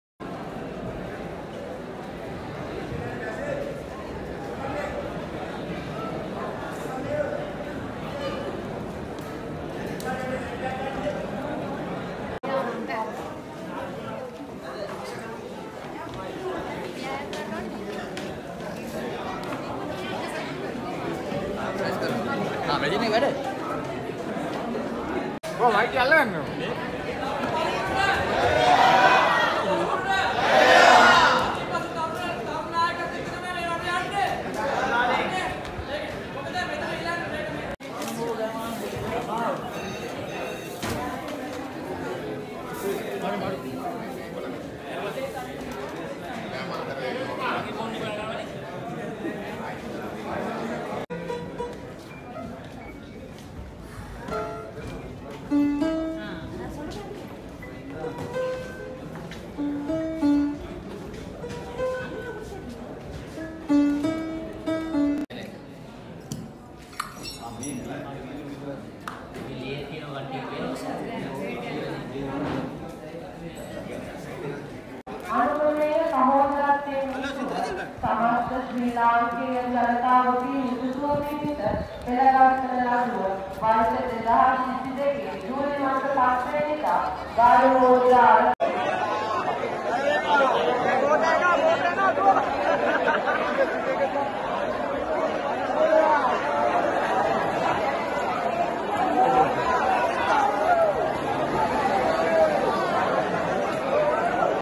Sri Lankans explore the presidential palace a day after storming
Members of the Sri Lankan public explored the presidential palace in Colombo a day after the official residence was stormed by protesters.